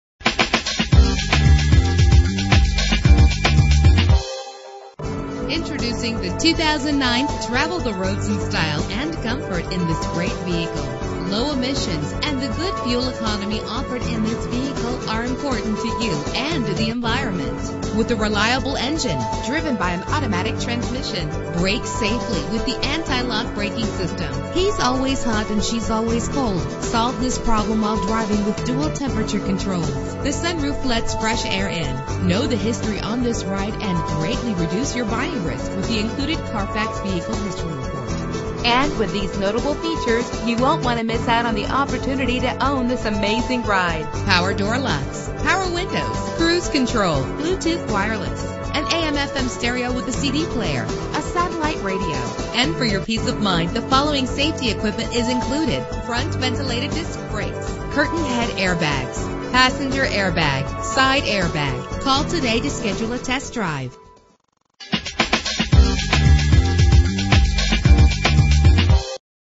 2009 Mercedes-Benz C300 - Fairfield NJ
Year: 2009
Make: Mercedes-Benz
Model: C300
Trim: C-Class
Engine: Dual Fuel
Transmission: Automatic
Color: Steel Grey
Mileage: 21355
Address:
1230 Bloomfield Avenue